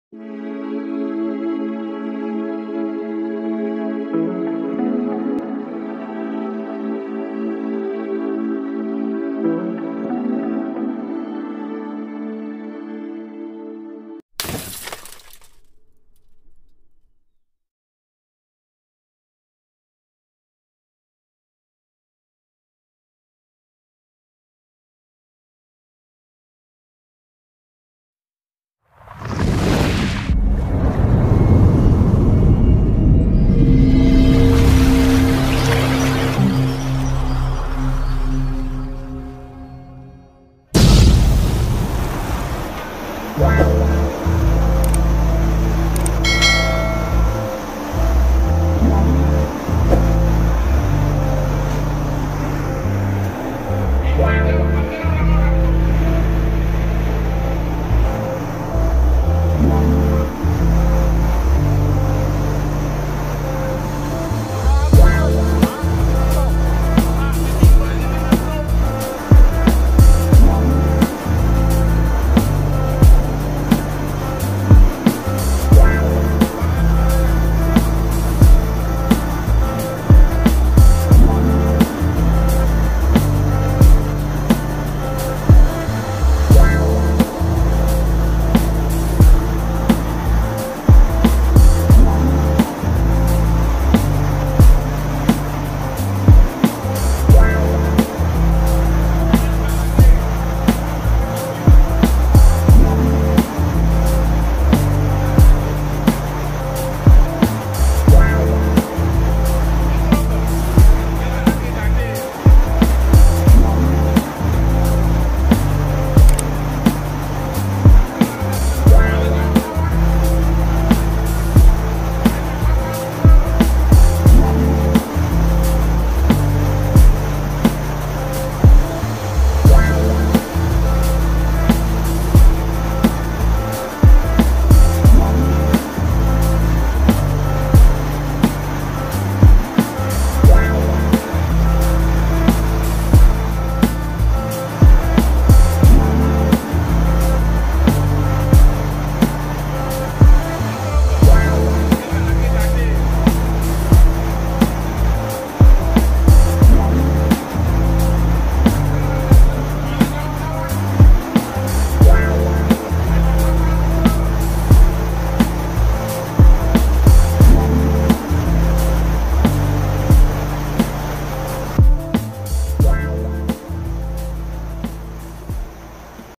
Kamar Mesin TB.Bintang mutiara 23 jadi kolam renang | kerja kapal
Diduga karna kebocoran kapal,enginee room TB.Bintang Mutiara 23 direndam air laut.Semoga Tidak ada korban jiwa

Silahkan Dukung Channel ini ya..

Terimakasih 🙏